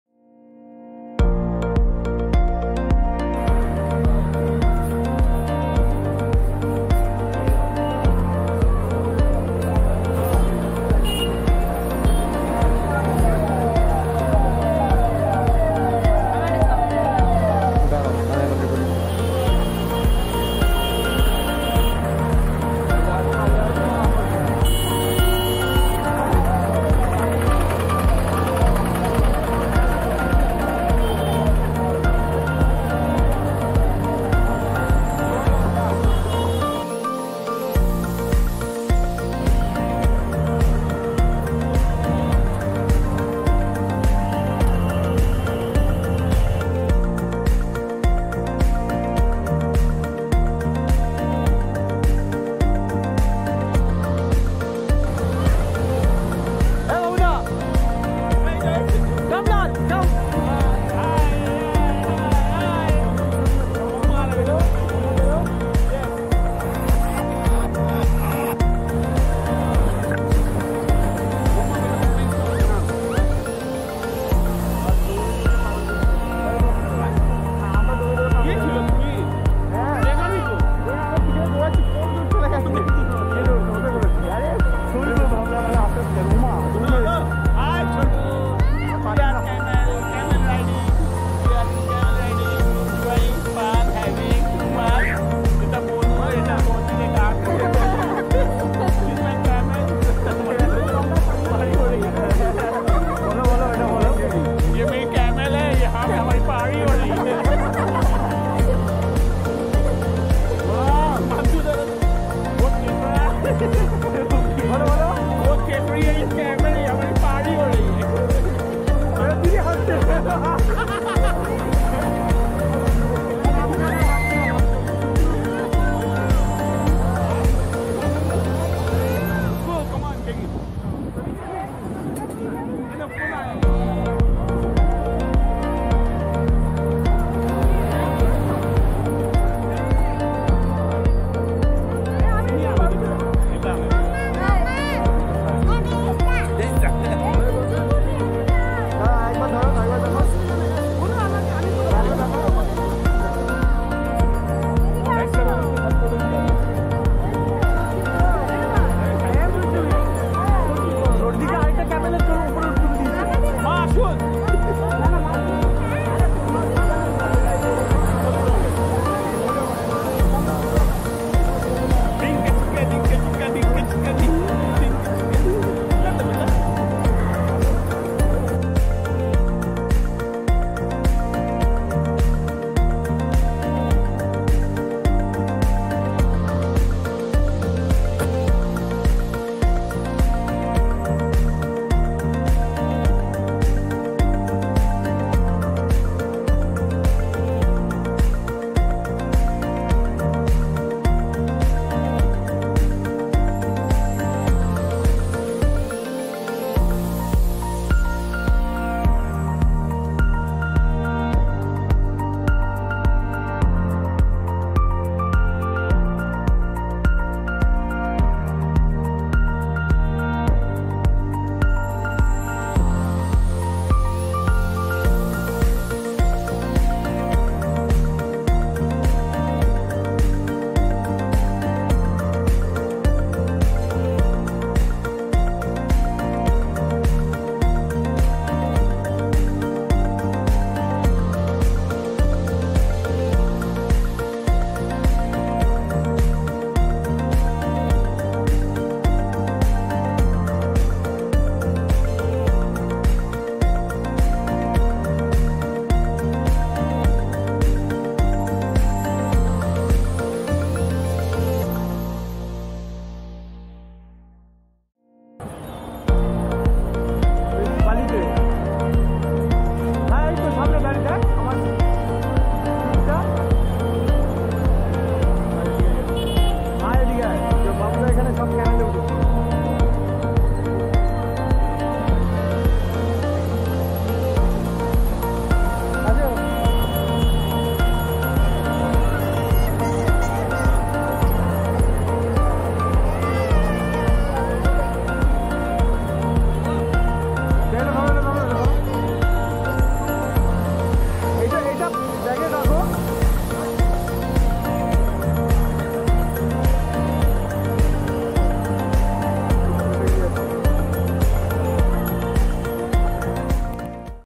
PURI TRIP WITH FAMILY❤️ || 4K ULTRA HD
CAMEL 🐪 RIDE ON THE BEACH OF PURI❤️Insta360 one R.